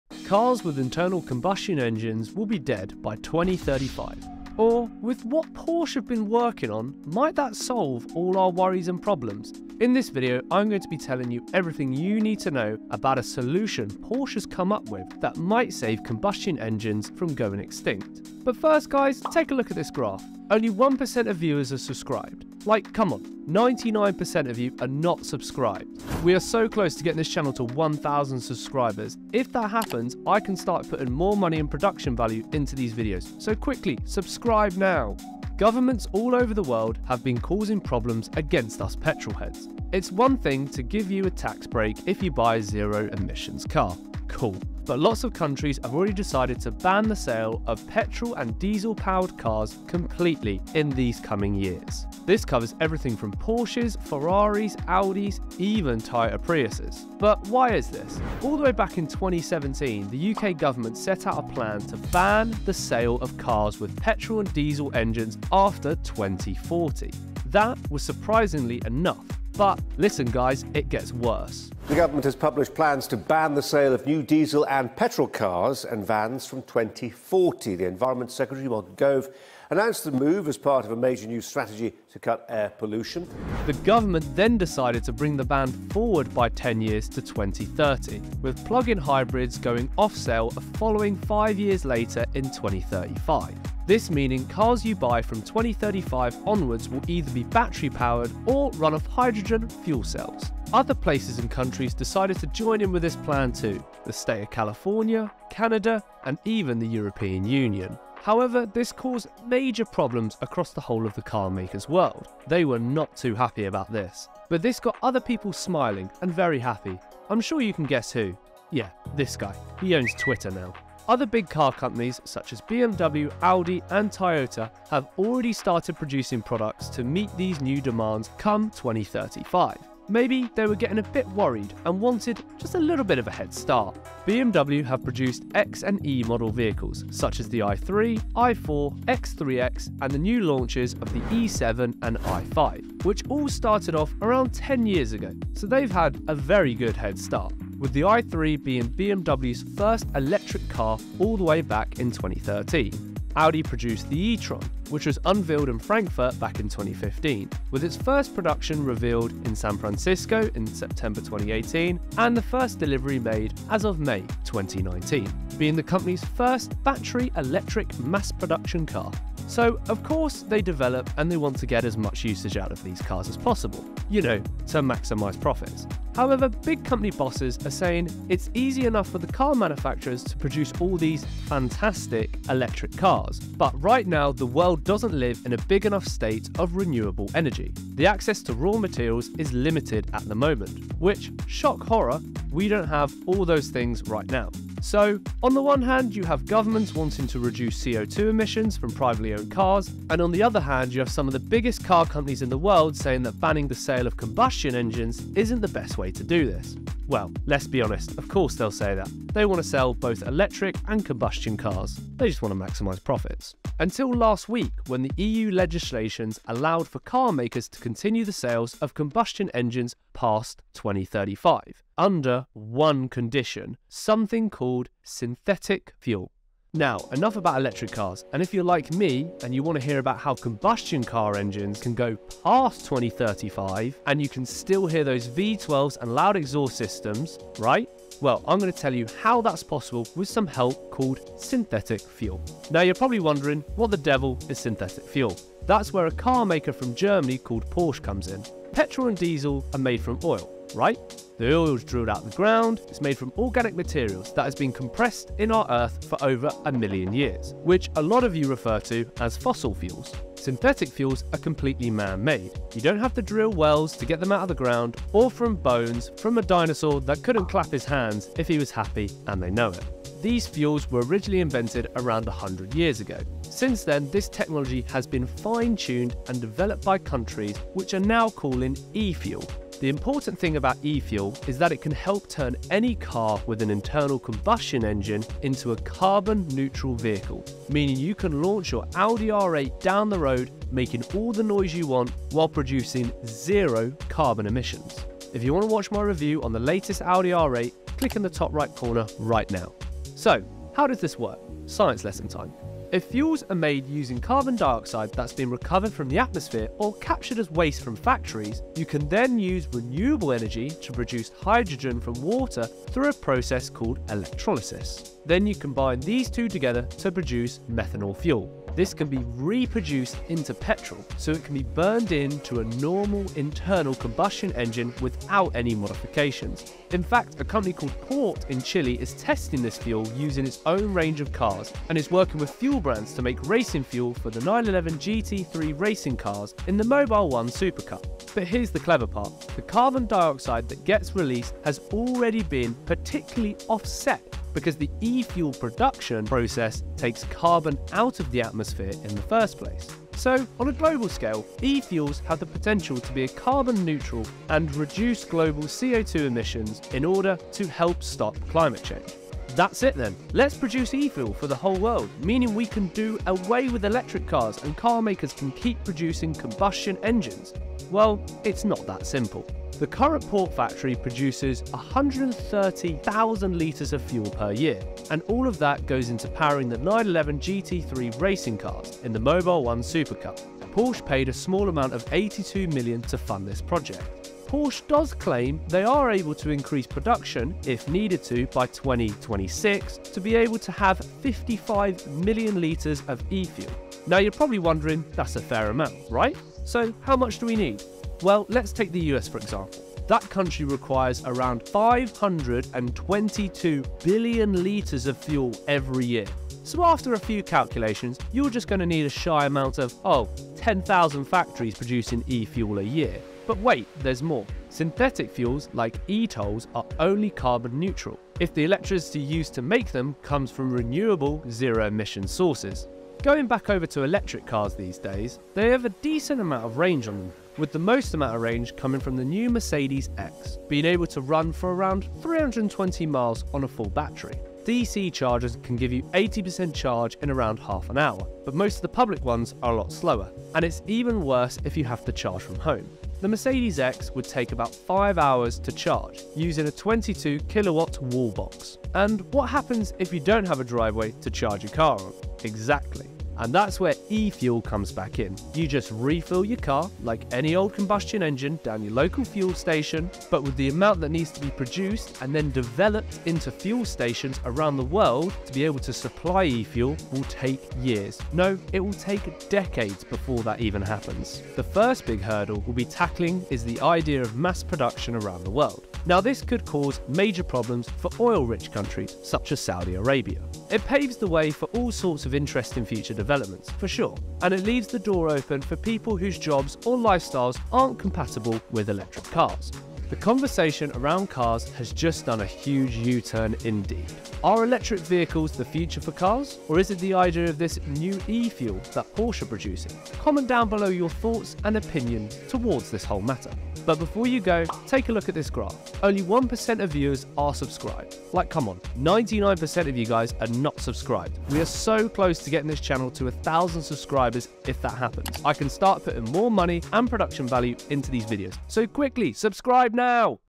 Porsche Has Saved PETROL Engines FOREVER
Cars with internal combustion engines will be dead by 2035 or with what Porsche has been working on might solve all our problems. In this video I am going to be telling you everything you need to know about a solution Porsche have come up with that might save combustion engines from going extinct.

_________________Content Chapter__________________

00:00 - Intro
00:35 - Future Laws on Combustion Engines
01:48 - How Does This Affect Major Car Companies
03:46 - What is Synthetic Fuel?
05:00 - How Does it All Work?
06:09 - Where does it All Come From?
08:10 - Outro

_________________Footage Credit____________________